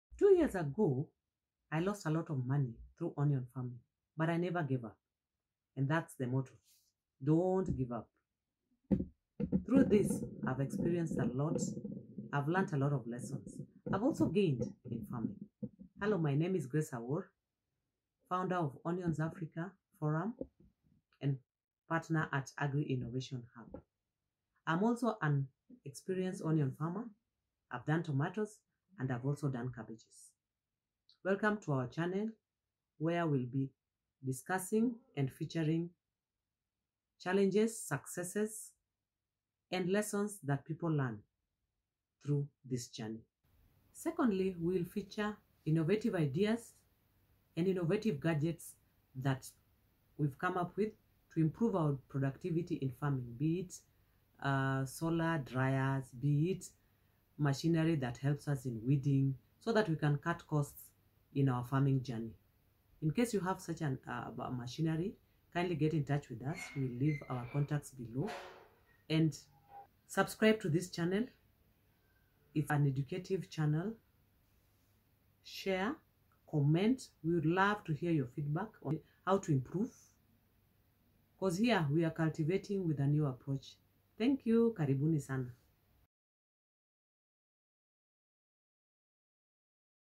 WELCOME TO OUR YOUTUBE CHANNEL (INTRODUCTION)
Am Grace Awuor,  proprietor Onions Africa and partner at Agri Innovation Hub, am an onion, tomato, cabbage farmer.
We shall feature simple innovative ideas eg machinery that help farmers improve on their productivity, so that we can save a lot on labor costs. So incase you have any these innovations, farming gadget, or ideas kindly get in touch with us, will we will leave a link below. comment, share and lets make this journey an easy one. Cause here we are Cultivating With a New Approach